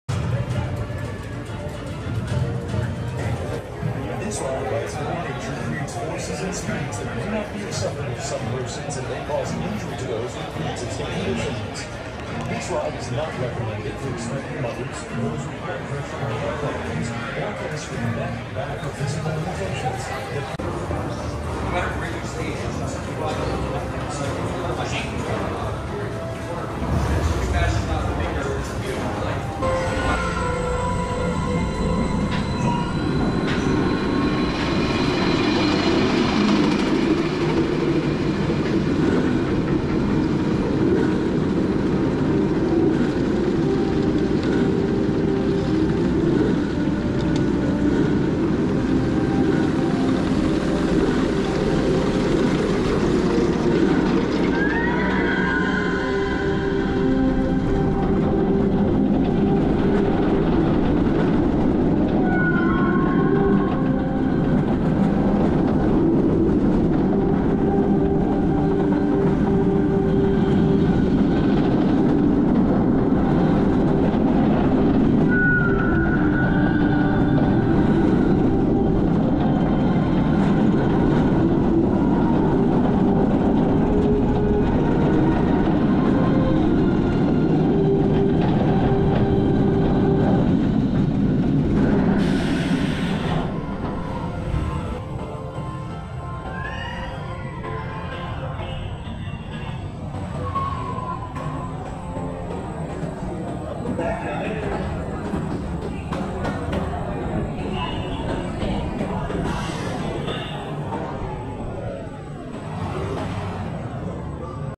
Phoenix Rising • Busch Gardens Suspended Family Roller Coaster | Queue, Load Station and Ride POV
A suspended family roller coaster at Busch Gardens Tampa Bay, Phoenix Rising. This coaster goes 44 miles per hour, reaches a height of 80 feet, has a track length of 1,831 feet and features lots of fun air times and quick maneuvers.

0:00 Ride Entrance and Queue
0:20 Ride Station
0:30 Phoenix Rising front-row POV